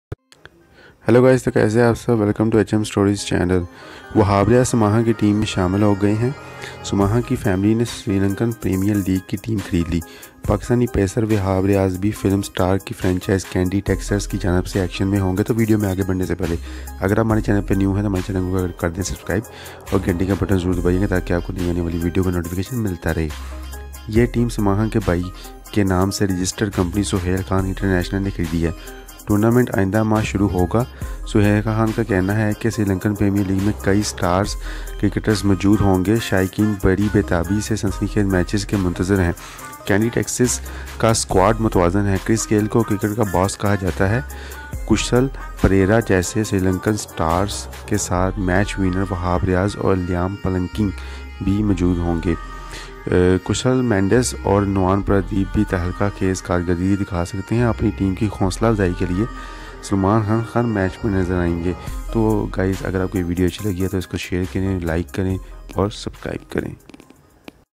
Wahab Riaz Join Salman Khan Team at SPL | Sri Lanka Premier League 2020
Wahab Riaz is a Pakistani cricketer. He is a left-arm fast bowler and a right-handed batsman.His all round performance in ICC Cricket World Cup 2015 earned him worldwide recognition. Now he Join the Salman Khan team at Sri Lanka Premier League 2020.